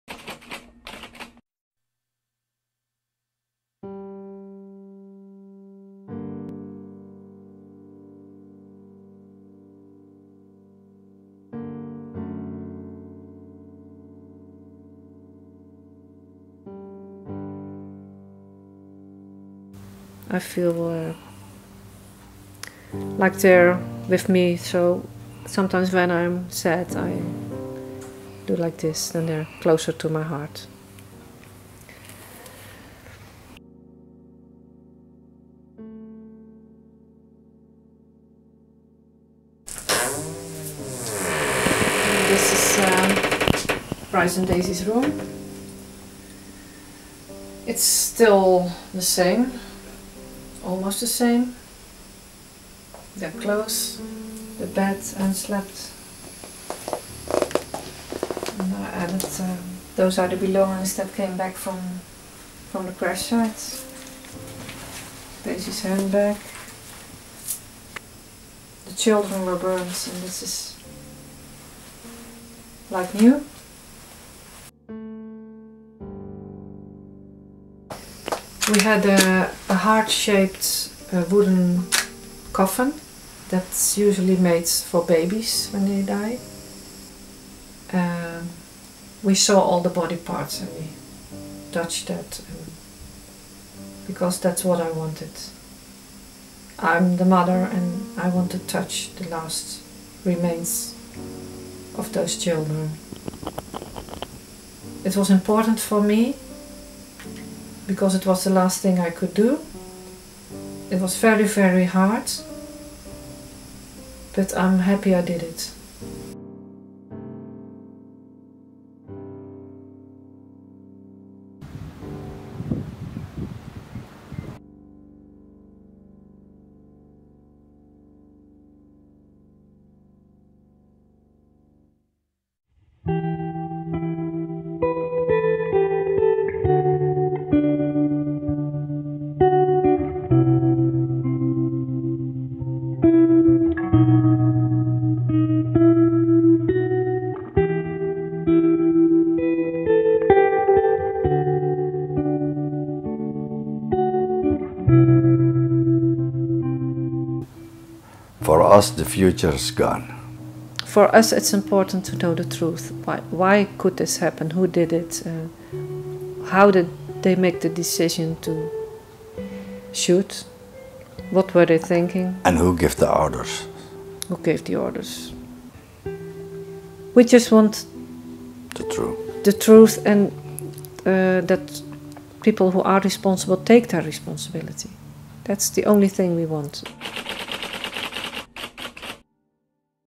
Silene and Rob Fredriksz lost their son and his girlfriend in MH17 tragedy
Photos of a young and smiling couple are almost everywhere in the house of Silene Fredriksz and her husband Rob. Pictured are their 23-year-old son Bryce and his girlfriend Daisy. They died on July 17, 2014, when a Russian Buk missile shot down a passenger plane performing flight MH17 from Amsterdam to Kuala Lumpur in the sky over Donbas. The tragedy killed 283 passengers (including 196 Dutch citizens) and 15 crewmembers. An international investigation concluded that MH17 had been shot down from a Buk missile system that belongs to the 53rd Anti-Aircraft Missile Brigade of the Russian Armed Forces stationed in Kursk. Russia denies its involvement in the downing of the plane.